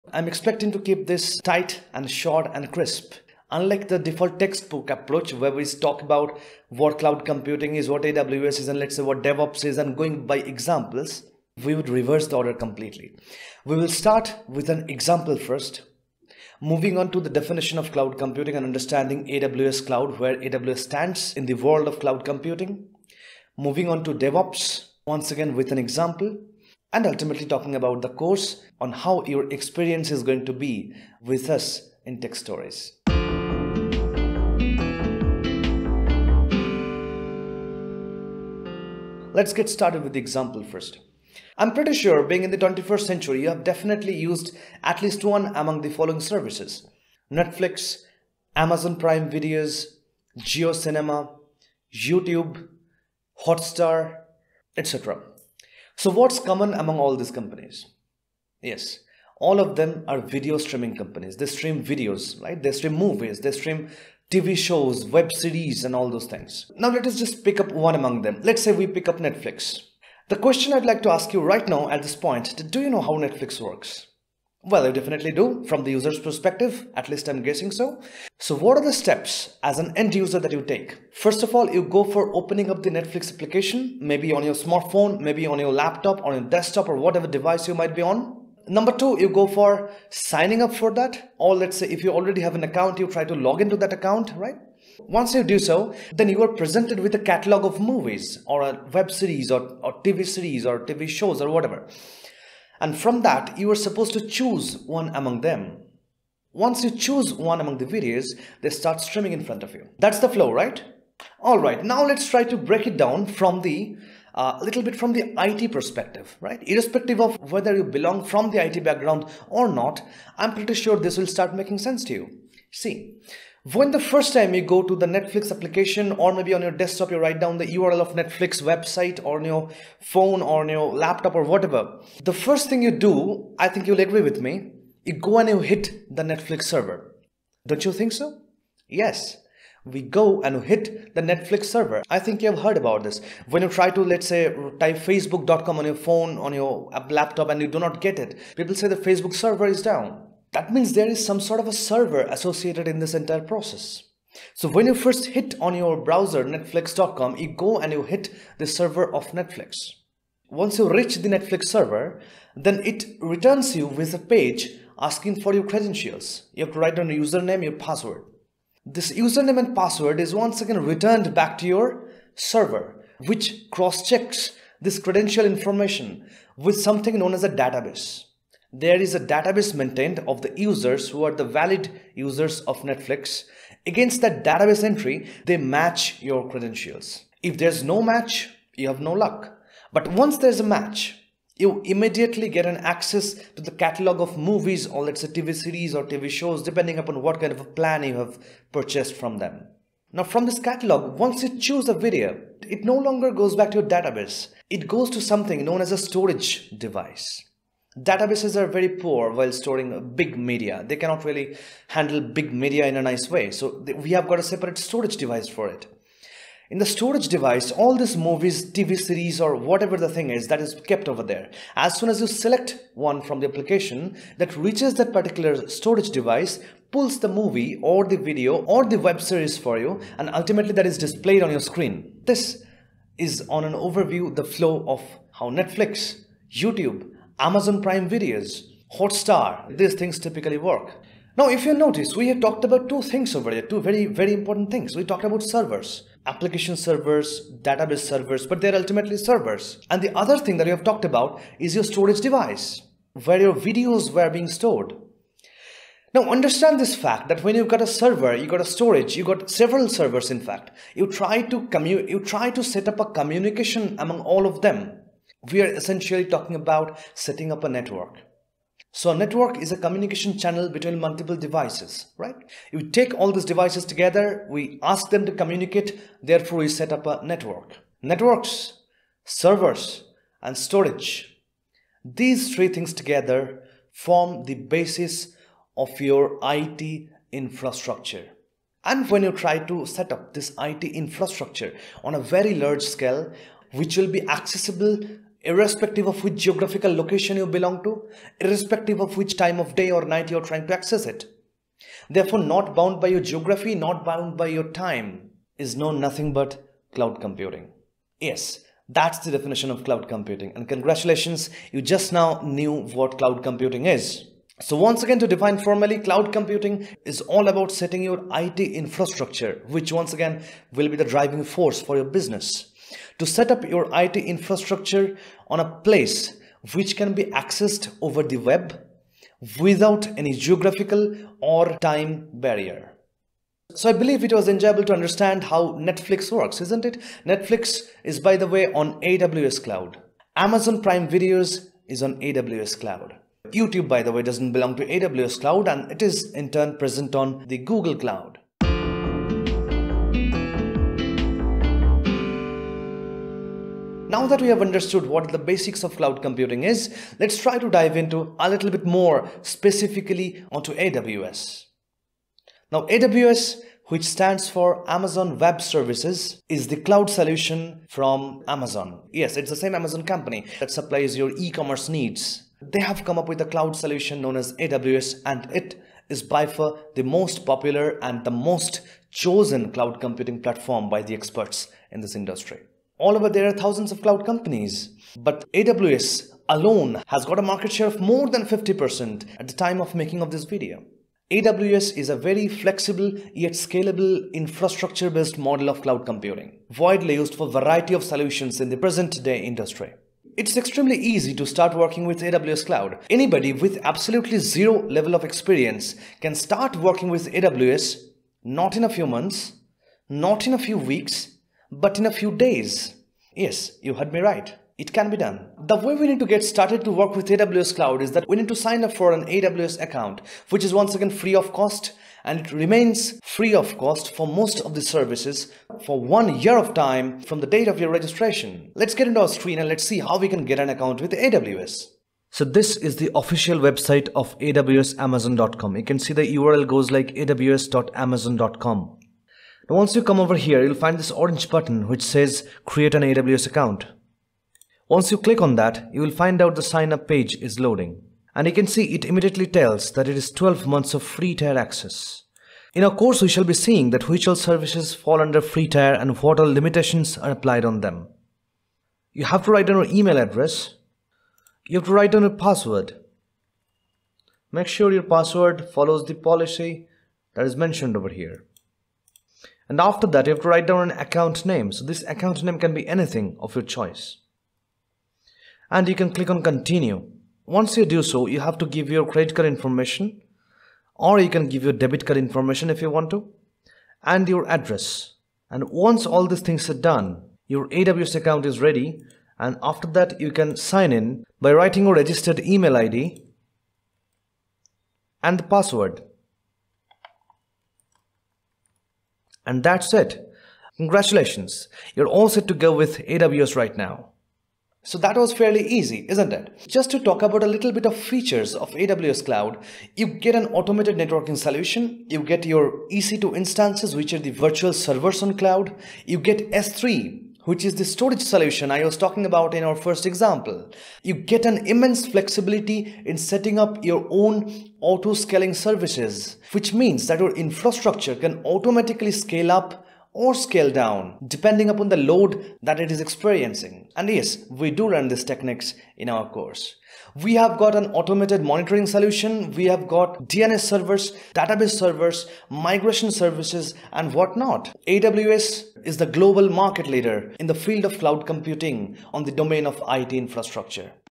Understanding Cloud Architect with AWS - Teqstories
Welcome to the demonstrative session on AWS Solutions Architect by Teqstories. This video is made to give you a clear understanding of AWS Solutions Architect and explains why it is one of the most trending technologies currently. The video also helps one in understanding how AWS is being implemented in the current industry and how it is going to be the future of IT world. It also covers what are the services which one needs to understand in order to be an AWS Architect.